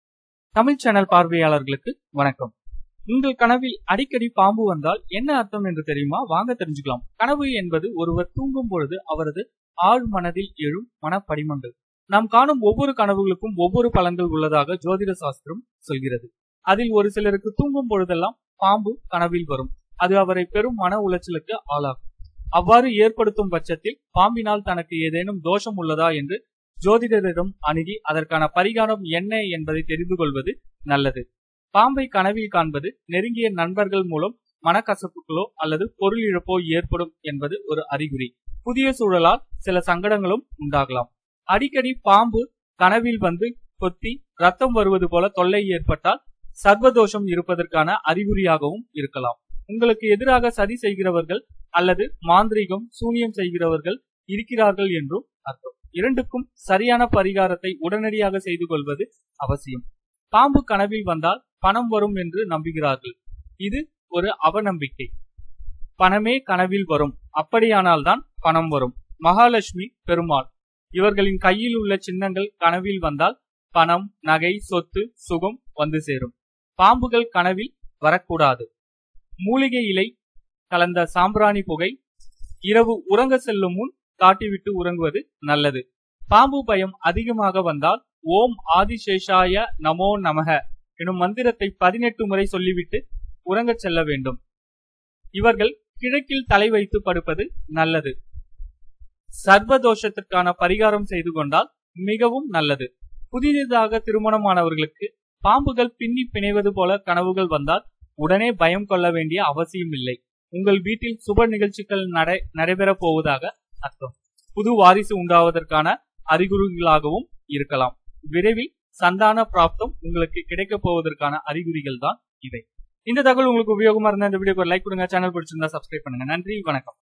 உங்கள் கனவில் அடிக்கடி பாம்பு வந்தால் என்ன அர்த்தம் என்று தெரியுமா?
உங்கள் கனவில் அடிக்கடி பாம்புகள் வந்து தொல்லை கொடுக்கின்றதா?